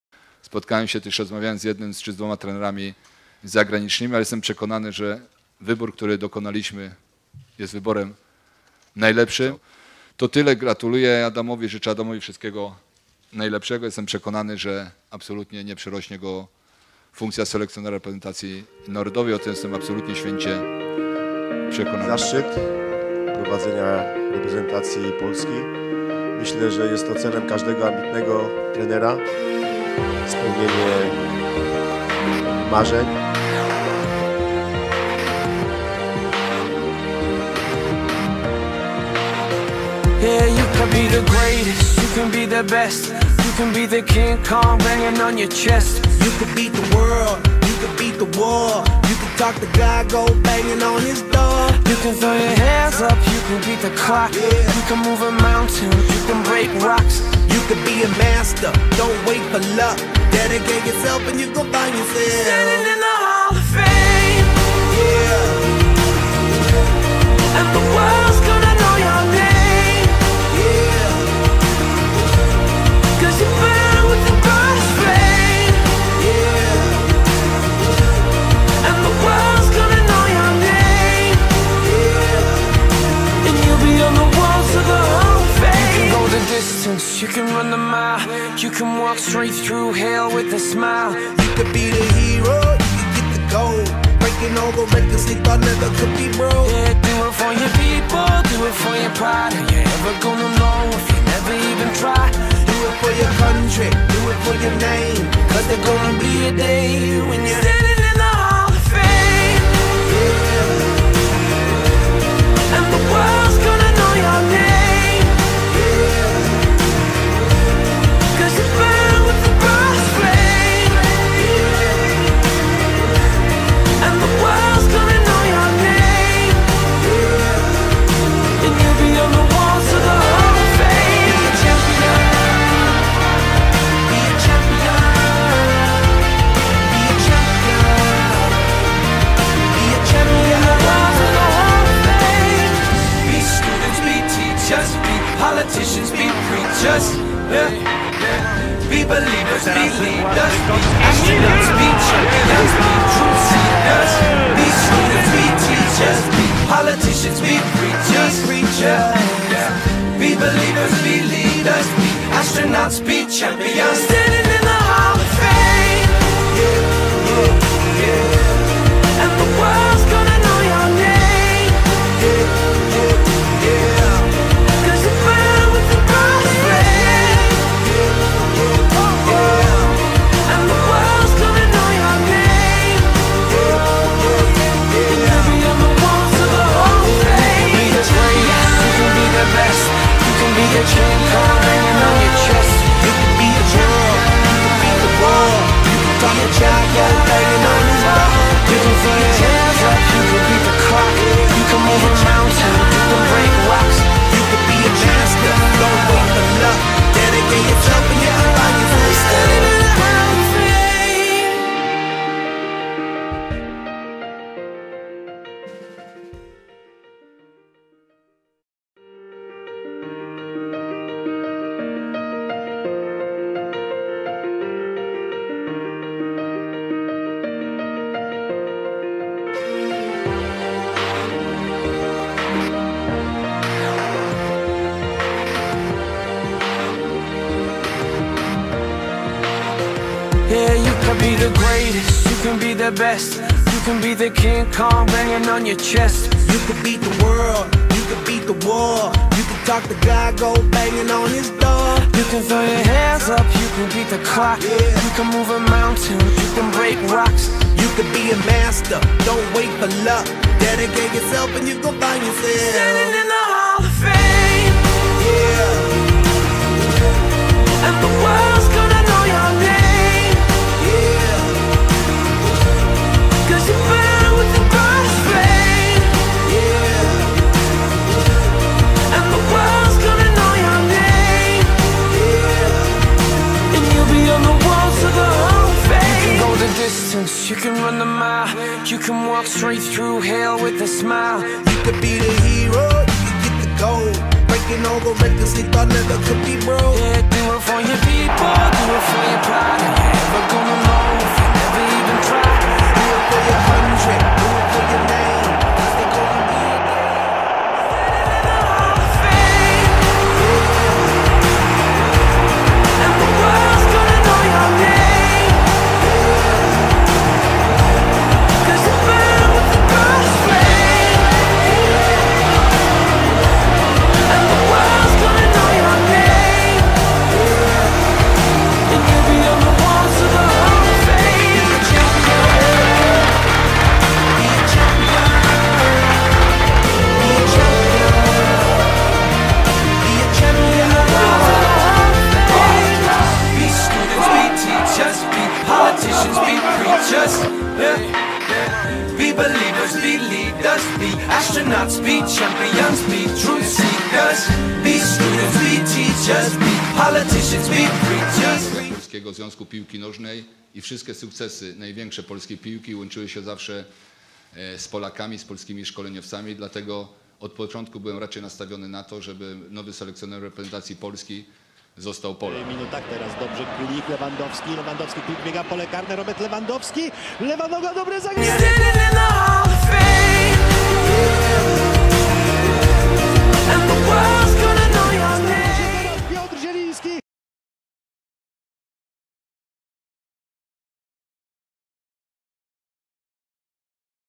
Adam Nawałka - Remember / Wspomnienie Najlepszego Trenera Polski 21 Wieku | 2018
Witam, emocję po mundialowe całkowicie opadły także zabrałem się za film pożegnalny trenera Nawałki i przedstawiam najlepsze momenty Adama z reprezentacją, miłego seansu.